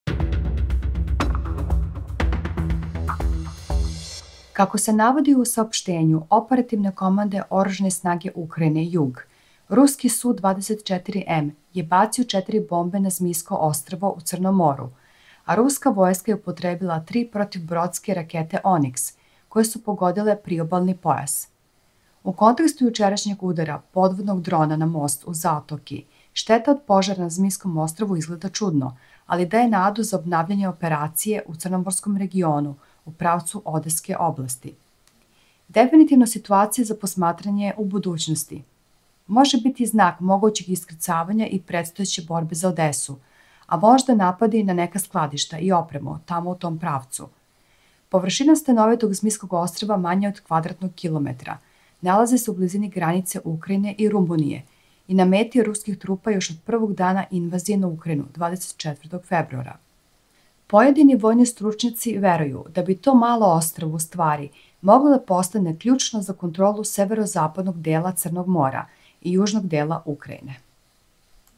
Rusija I Ukrajina Rat najnovije Vesti ,Rusi "Onixom" udarili na Zmijsko ostrvo
O udarima na Zmijskom ostrvu i upotrebi „Oniksa“ Kako se navodi u saopštenju operativne komande OSU „Jug“, ruski Su-24M je bacio četiri bombe na Zmijsko ostrvo u Crnom moru, a ruska vojska je upotrebila 3 protivbrodske rakete „Oniks“ , koje su pogodile priobalni pojas.
U kontekstu jučerašnjeg udara podvodnog drona na most u Zatoki, šteta od požara na Zmijskom ostrvu izgleda...čudno ali daje nadu za obnavljanje operacija u crnomorskom regionu u pravcu Odeske oblasti.
Definitivno situacija za posmatranje u budućnosti. Može biti i znak mogućeg iskrcavanja i predstojeće borbe za Odesu, a možda napadi na neka skladišta, i opremu tamo u tom pravcu...
Površina stenovitog Zmijskog ostrva manja je od kvadratnog kilometra. Nalazi se u blizini granice Ukrajine i Rumunije, a na meti ruskih trupa je od prvog dana invazije na Ukrajinu, 24. februara.

 Pojedini vojni stručnjaci veruju da bi to malo ostrvo u stvari moglo da postane ključno za kontrolu severozapadnog dela Crnog mora i južnog dela Ukrajine.
Info sa terena: Telegram kanal Dejana Berića